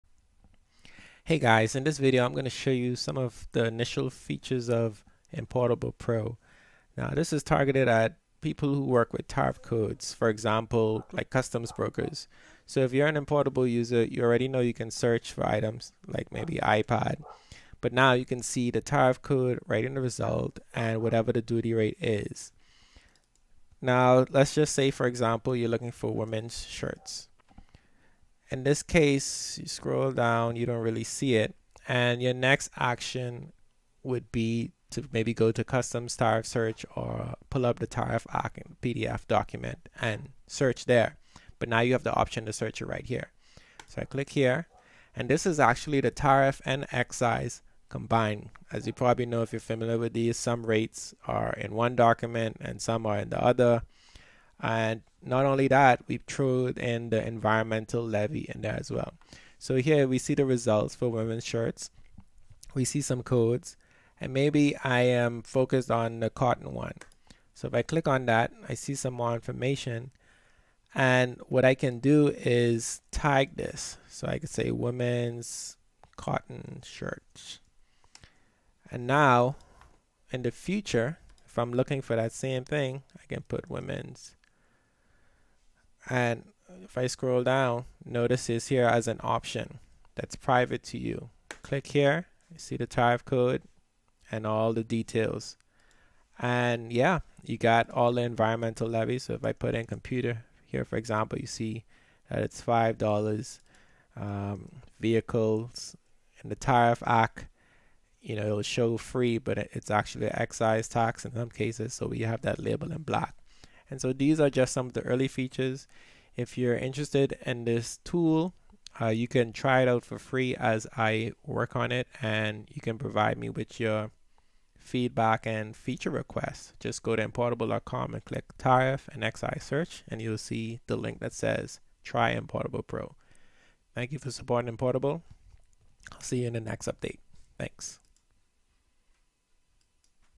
Bahamas Customs Tariff Search Alternative
Search tariff and excises in one place. Tag codes for future reference.